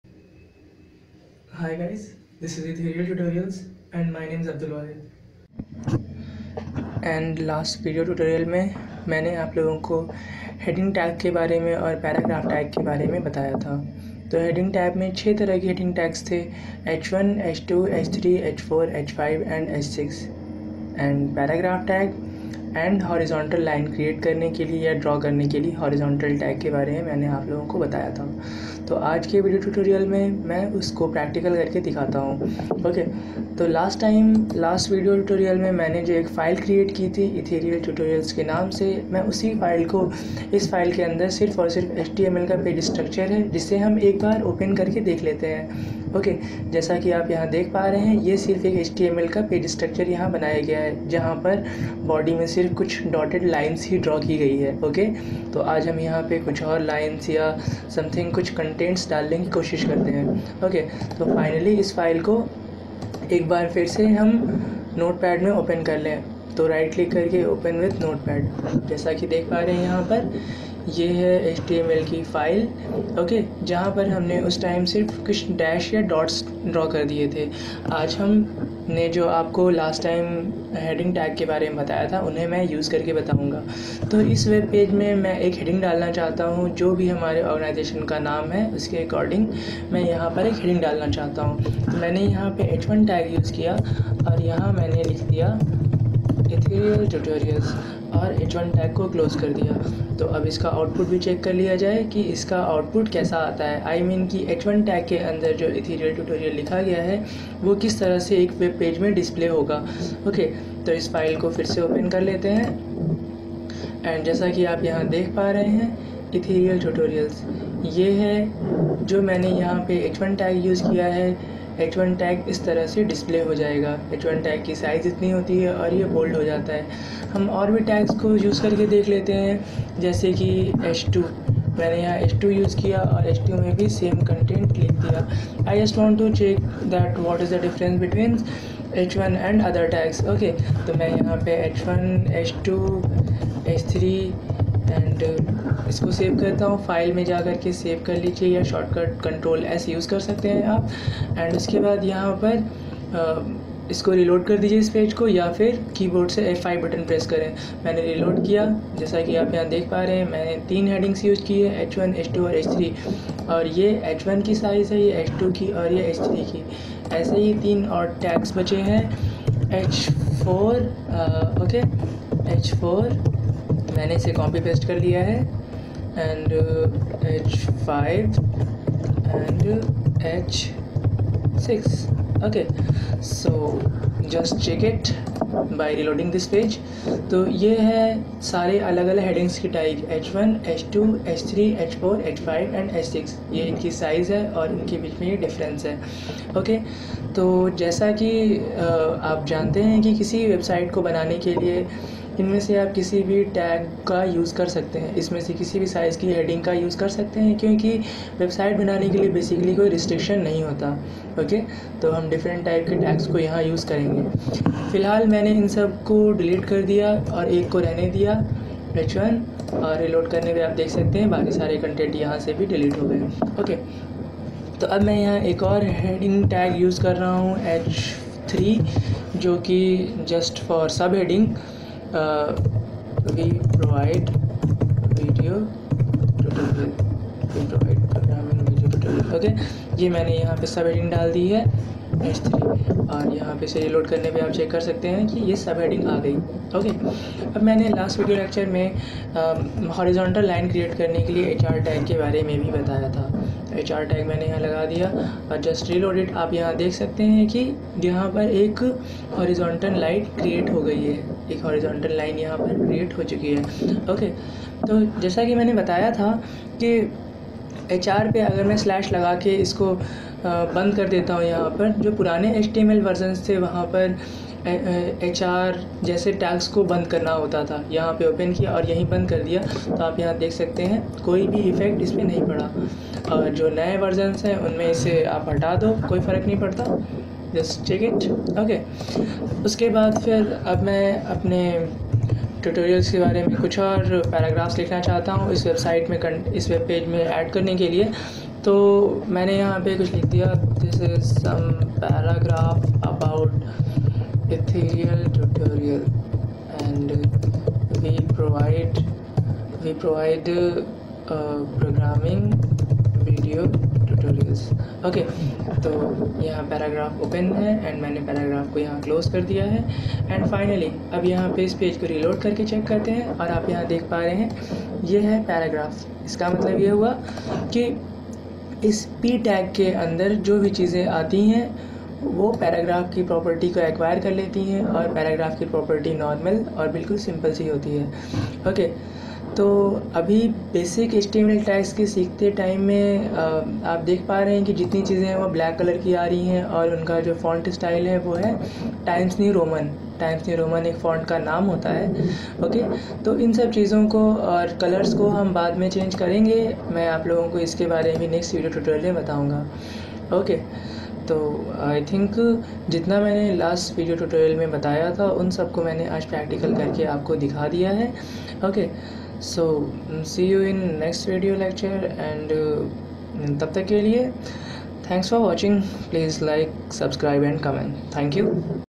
HTML heading and paragraph tags by practical-5
This tutorial contains explanation of Heading tags, paragraph tag and some other tags through practical